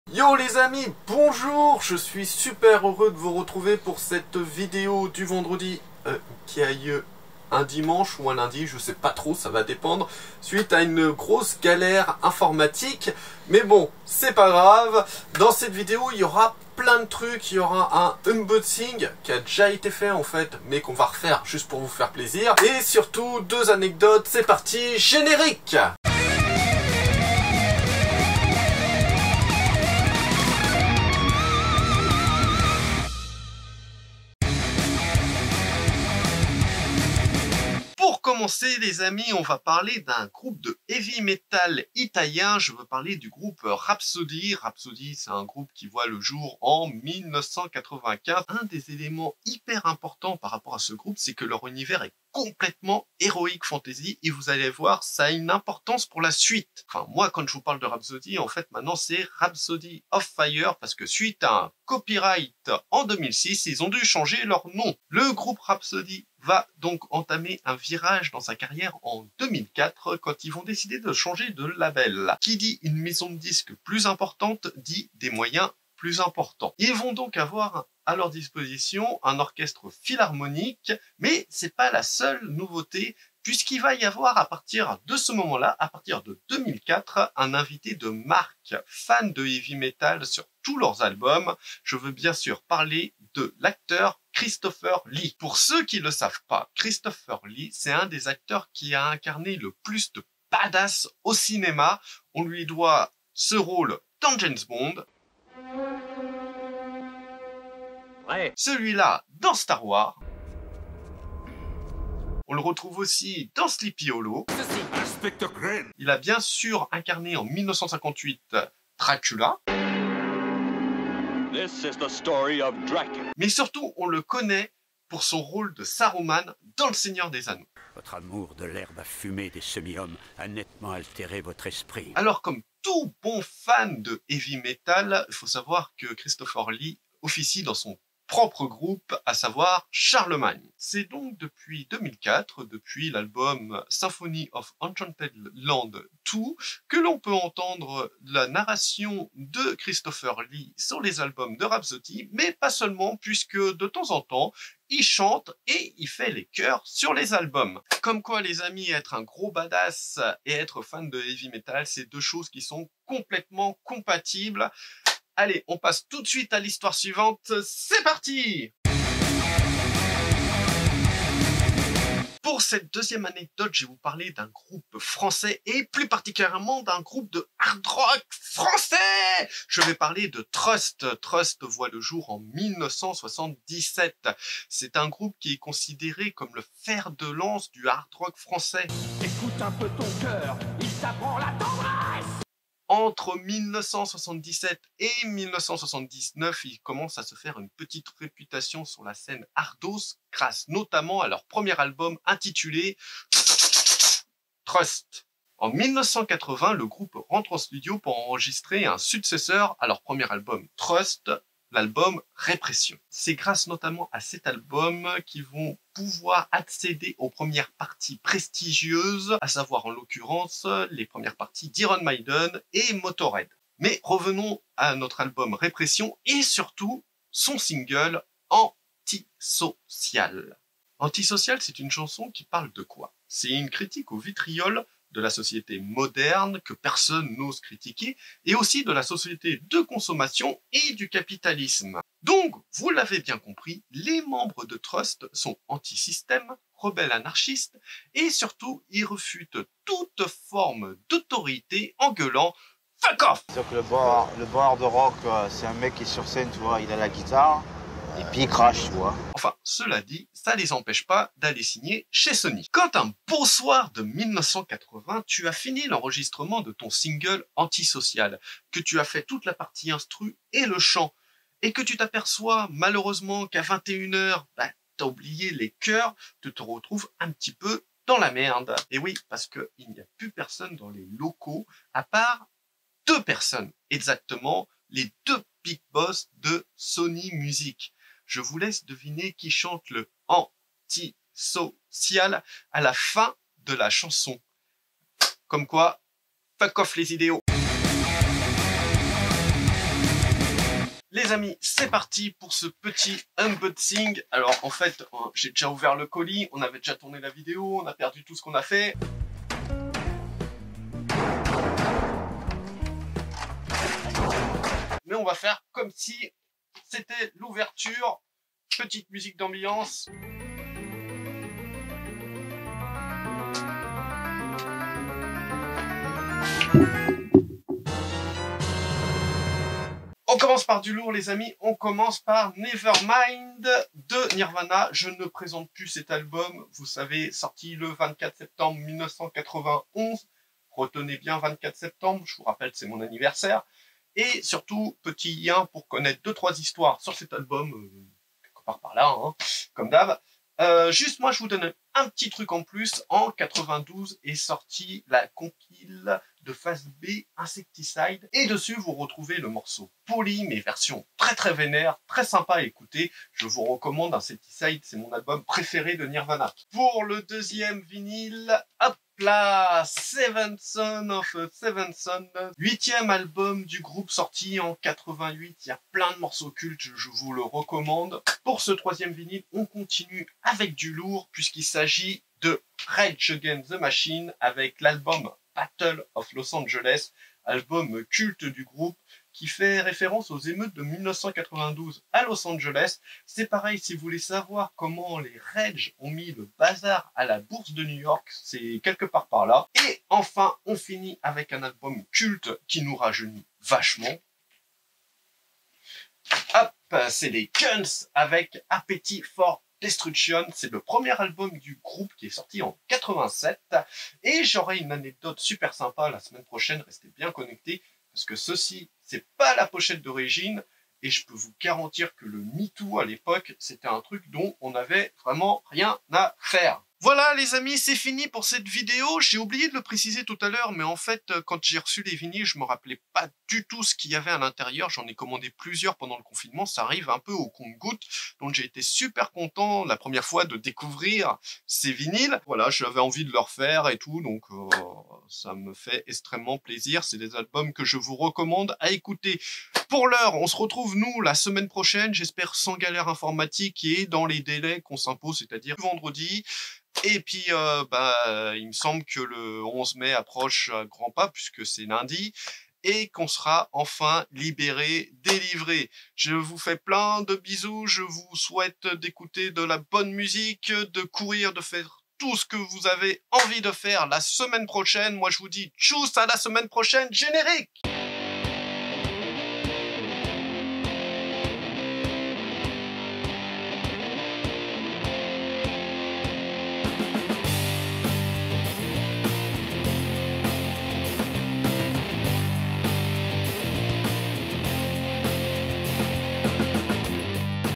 ROCK STORY : Rhapsody et Saroumane , Trust vs Sony + unboxing vinyle : Maiden, RATM, Nirvana, Guns
Yo les amis, comme prévu la vidéo du vendredi qui sort un dimanche car suite à un léger problème informatique et plus précisément une réinstallation de ce maudit windows. Alors oui, on a pas tenu le délai pour la deuxième semaine consécutive, mais nous avons tout fait pour vous fournir une vidéo cette semaine est le résultat est là : 2 anecdotes bien cool dont la rencontre entre le heavy métal et le seigneur des anneaux, ainsi que le monde magique des majors musicals et des hardos. Petit bonus on vous fait un unboxing vinyles de trucs dont on se souvient vaguement avoir commandés qui arrive au compte gouttes, pour, lors du déballages avoir de bonnes surprises. Bon je vous cache pas que l'unboxing on l'avait déjà fait lors de notre premier essai pour cette vidéo, mais comme on a perdu tous les rushs, on a du retourner cette vidéo, on a fait un unboxing du unboxing…
Il me reste juste à vous dire Tchussss les amis et à la semaine prochaine, normalement Vendredi si tout va bien sinon...bha un autre jour.

Matériel et logiciels utilisés pour cette vidéo
- Olympus E-M10 Mark iii
- Objectif Zuiko digital 14-42 mm